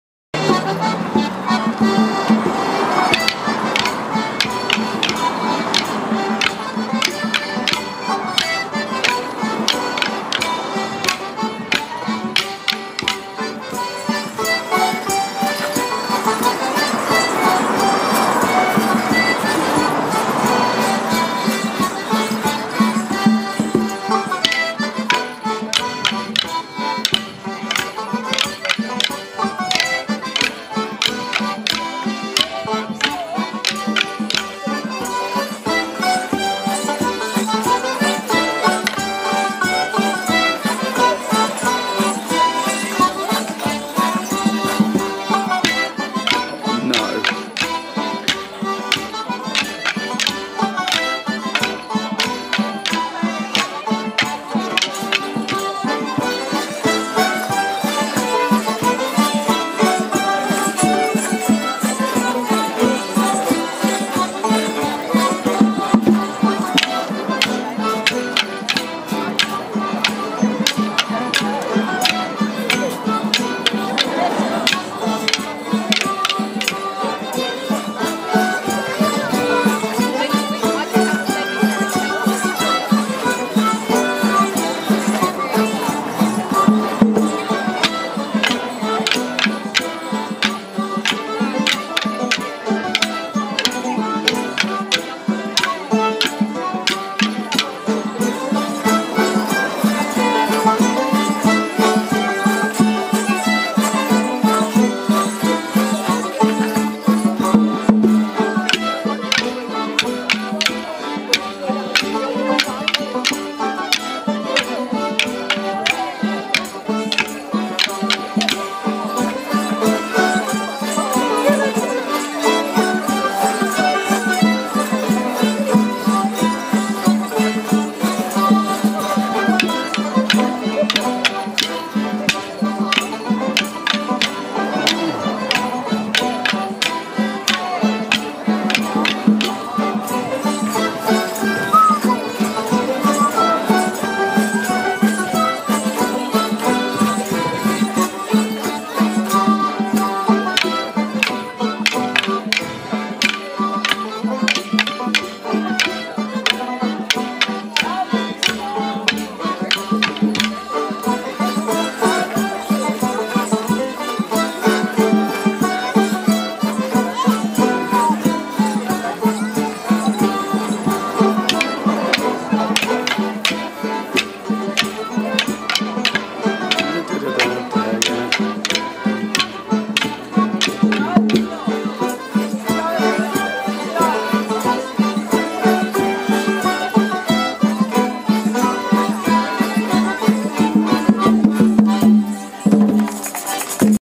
ELRiot Ely Stars Wicken 2016-05-01
The Ely and Littleport Riot dance Ely Stars at Wicken Parish Church on May Day 2016. Thanks to Steve Rule.